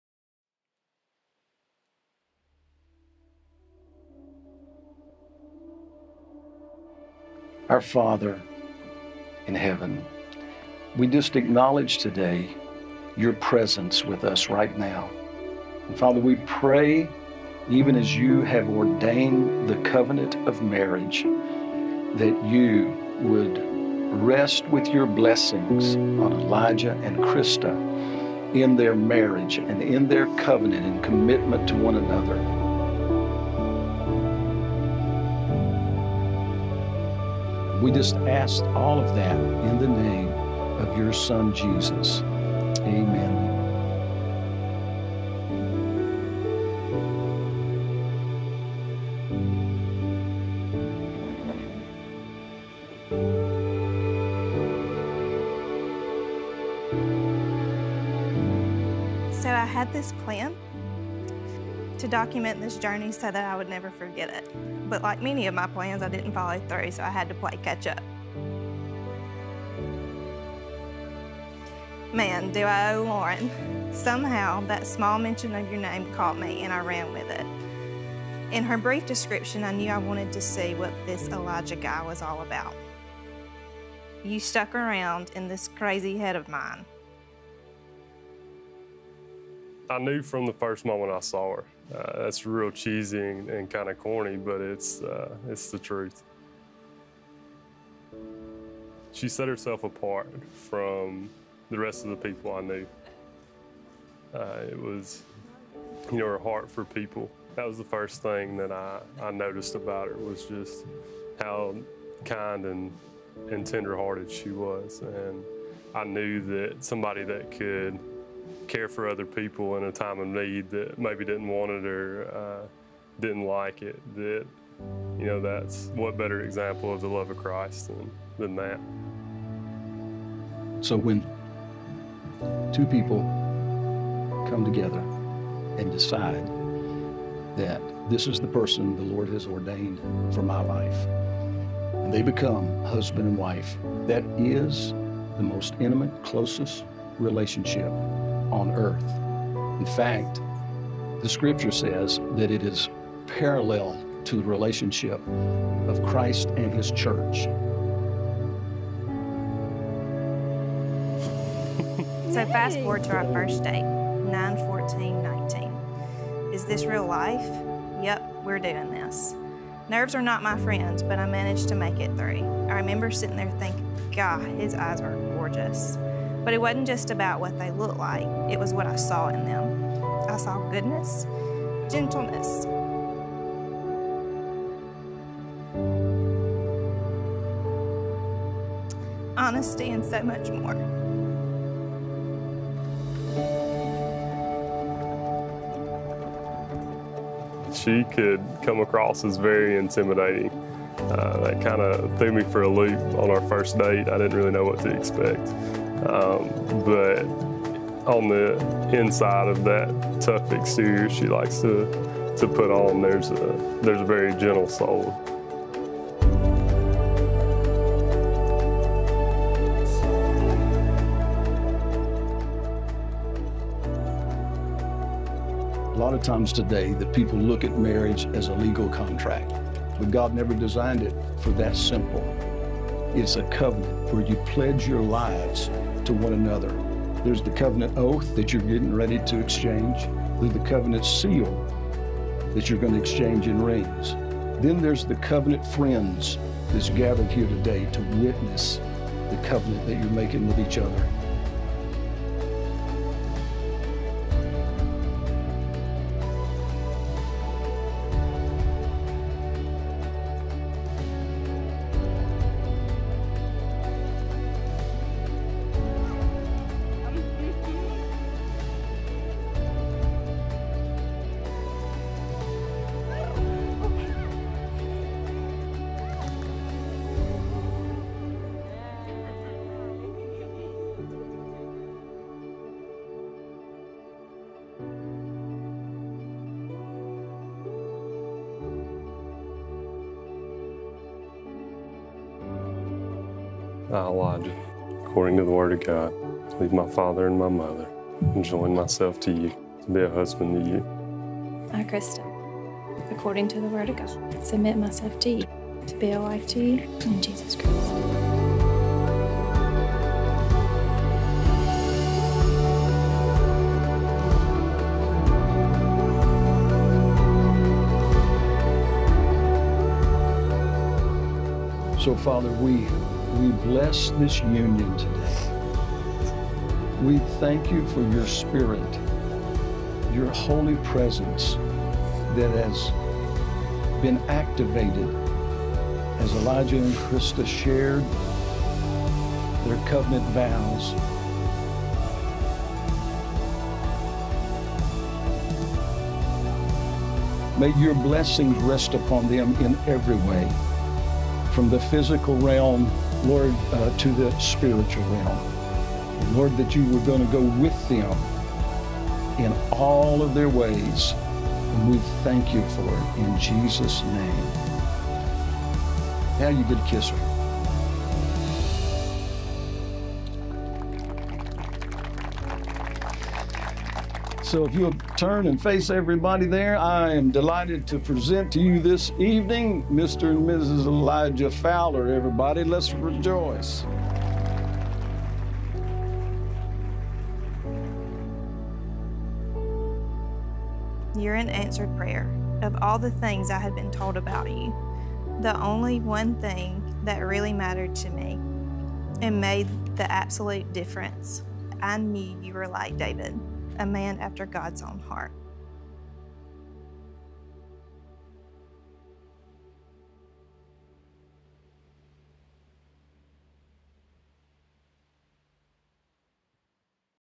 Bride Reads Emotional Diary Entry on Wedding Day | Krista & Elijah's Windgate Farms Wedding
Krista & Elijah's stunning Windgate Farms wedding in Jasper, AL was originally supposed to happen in the spring. Due to the Pandemic, these southern sweethearts adjusted their plans, and chose to get married privately, then seal that covenant in front of family and friends a few months later.

Krista surprised her groom with the diary she kept during their relationship-detailing how quickly and irrevocably she fell in love with him. Their story of love is one that is centered around their faith, and that faith is on display during every portion of their day.

We loved getting to serve these two on their day alongside all of these incredible vendors!

Alabama Wedding Vendors
Cinematography: Megan Pettus
Photography: Megan Kennedy Photography
Venue: Windgate Farms in Jasper, AL
Dress: The Bride & Groom, Columbus, MS
HAMU: Beauty Bar by Tori Crocker, Tuscaloosa, AL
Catering: Foxy Foods by Tracy Hardy
Florals: Eddie Brown & Beth Able
Band: SouthTown